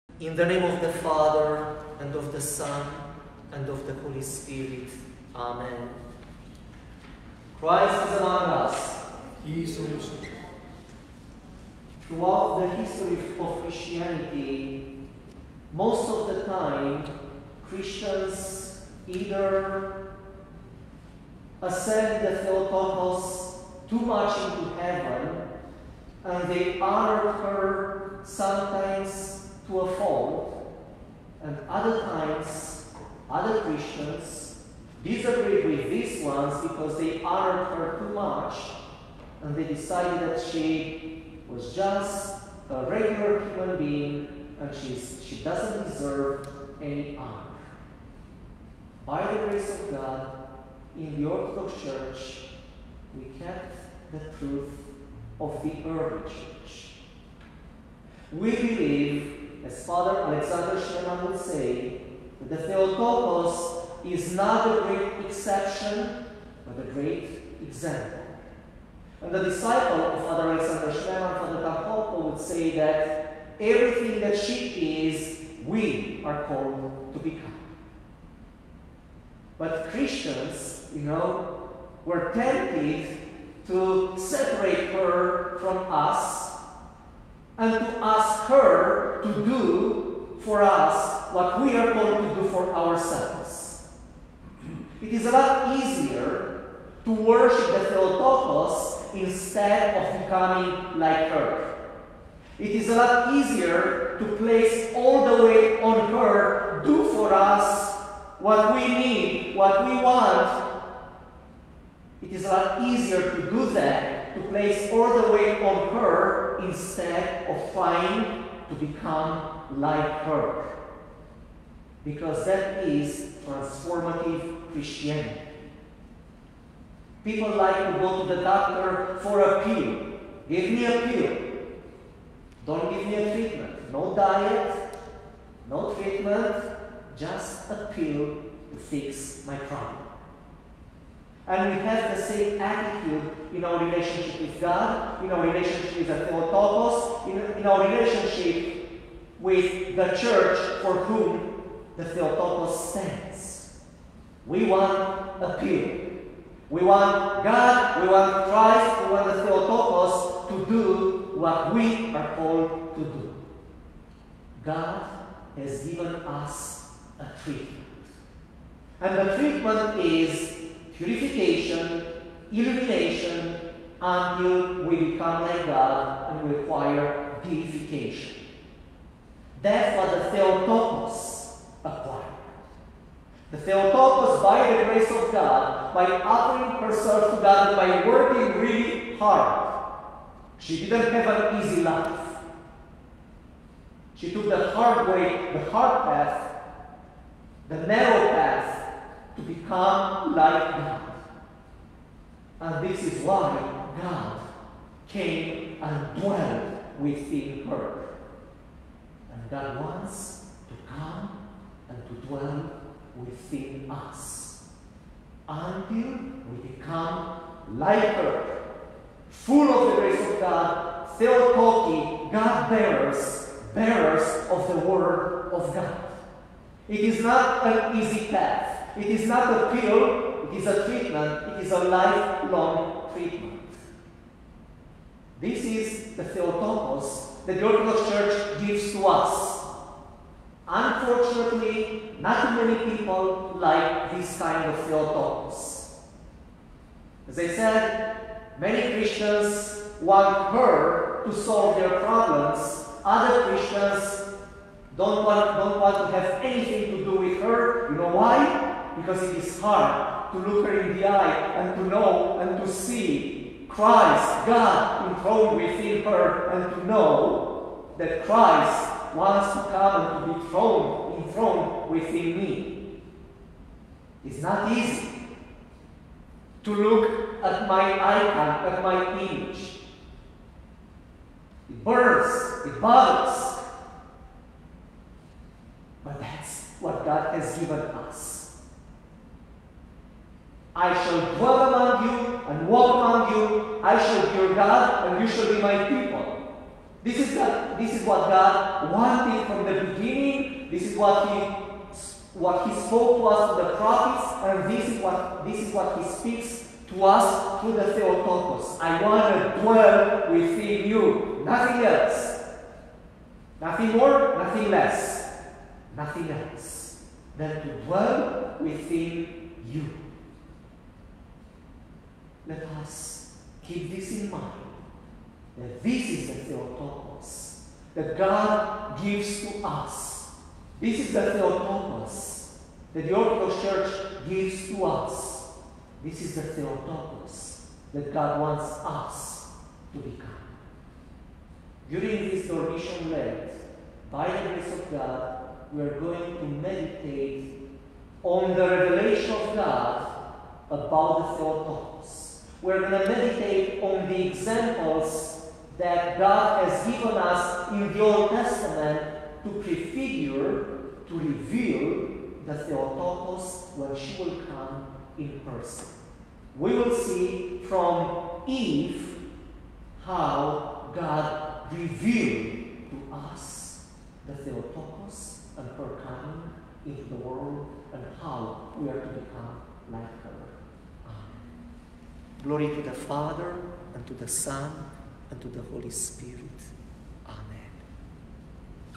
The Theotokos is the Great Example, not the Great Exception
Fr. Alexander Schmemann said: “Mary is not the great exception, but the great example”. In this first in a series of sermons about the Theotokos, we will see how Mary, the Mother of the Lord, is a transformative example for all of us.